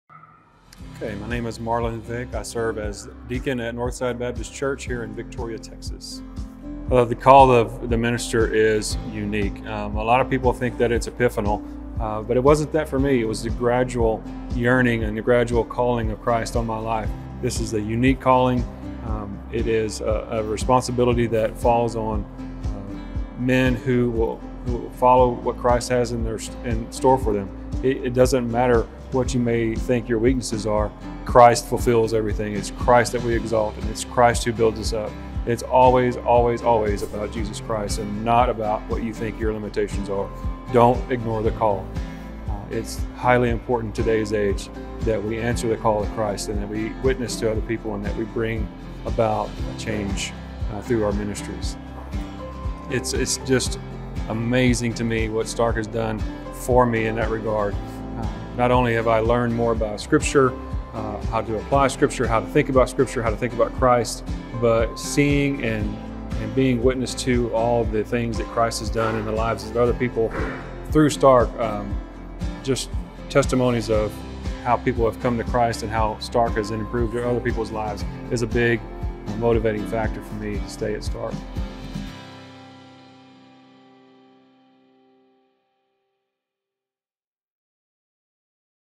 H&D2021 Marlon Vick
It’s never too late to go to Bible college! At Stark College & Seminary the average age of our students is 42 years old and they work full-time. Listen to Marlon Vick’s story of how the Lord brought him to SCS and how it has changed his life and ministry.